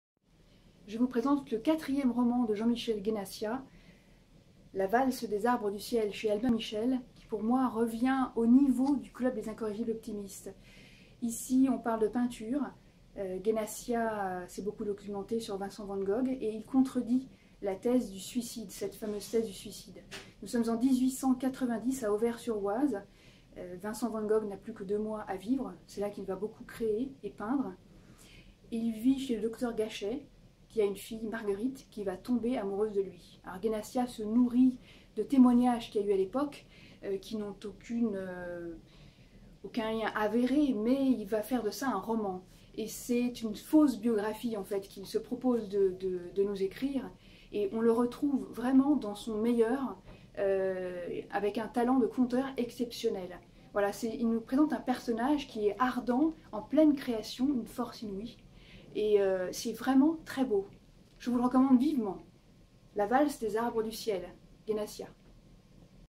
La valse des arbres et du ciel, de Jean Michel Guenassia
Le livre du jour, par Marie Joseph Biziou